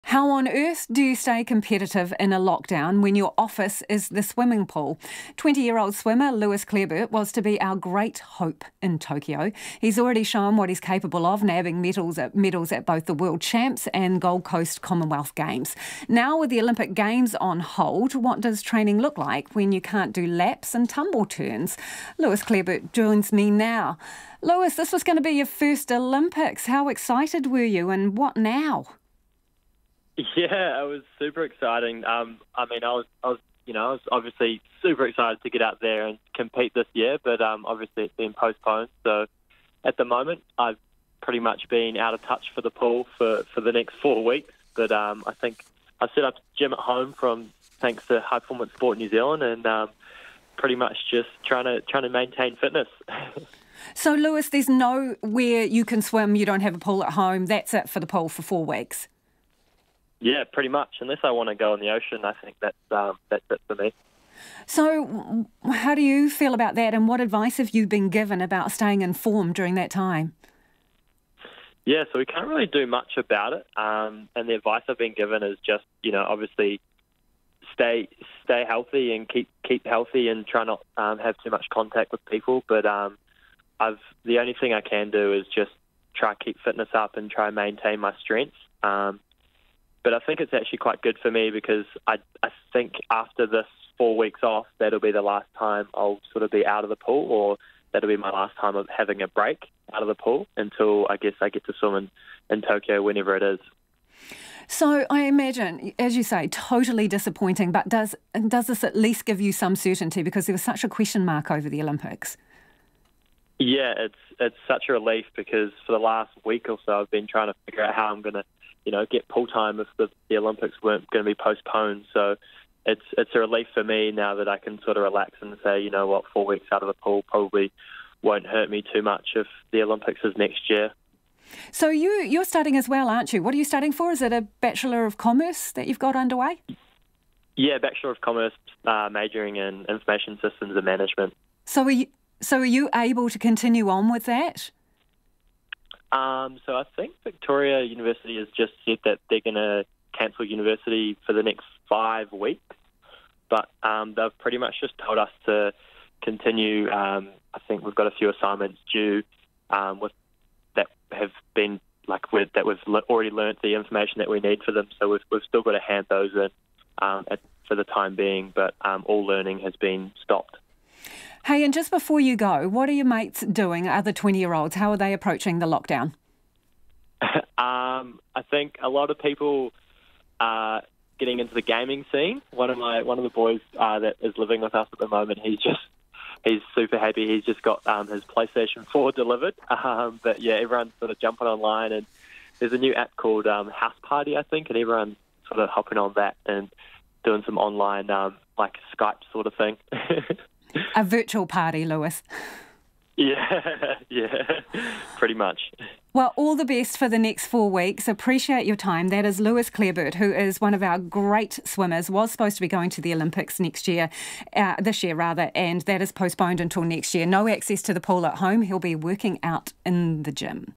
Olympics postponed, how do you train in lockdown?
How do you stay competitive in a lockdown when your office is the swimming pool?

Twenty-year-old swimmer Lewis Clareburt was to be New Zealand's great hope in Tokyo.

He has already shown what he's capable of - nabbing medals at both the world champs and Gold Coast Comonwealth Games.

Now with the Olympic Games on hold, what does training look like when you can't do laps and tumble turns? Lewis Clareburt talks to Lisa Owen.